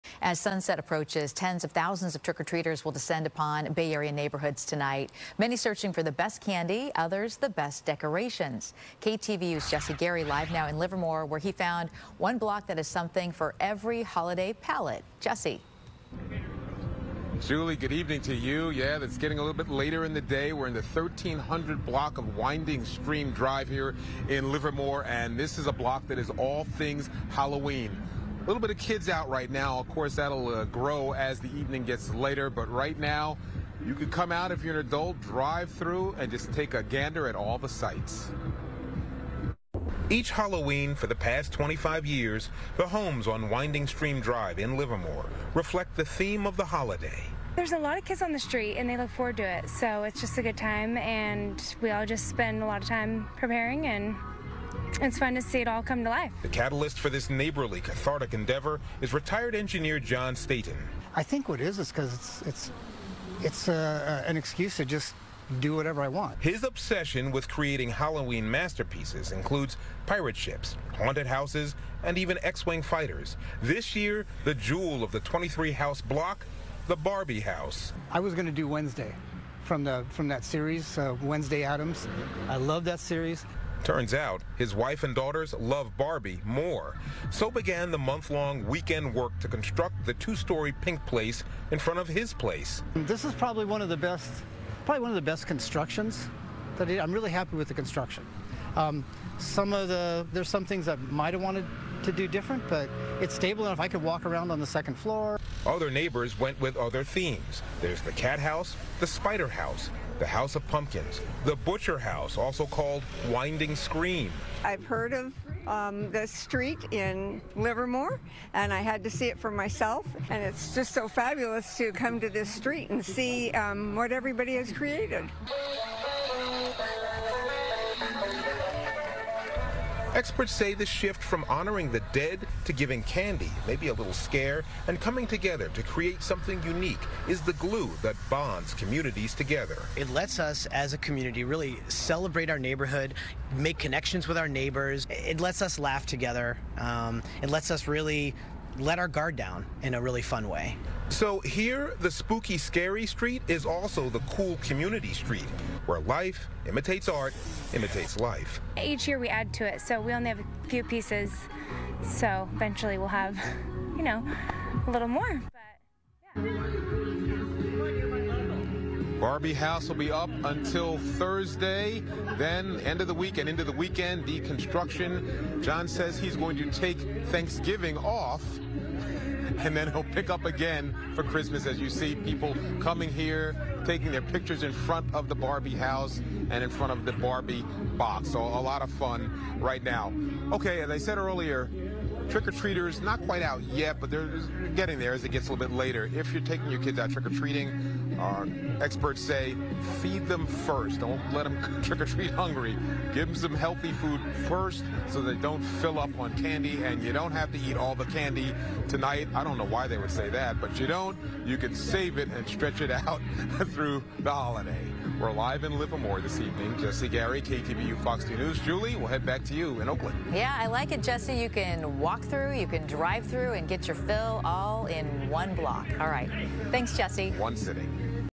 Barbie house takes spotlight on Halloween-obsessed Livermore block
Each Halloween, for the past 25-years, the homes on Winding Stream Drive in Livermore reflect the theme of the holiday.

"There’s a lot of kids on the street, and they look forward to it. So it’s just a good time. And, we all spend a lot of time preparing. And it’s fun to see it all come to life," said neighbor Lacey Stapenhorst.

The catalyst for this neighborly cathartic endeavor is retired mechanical engineer John Staton.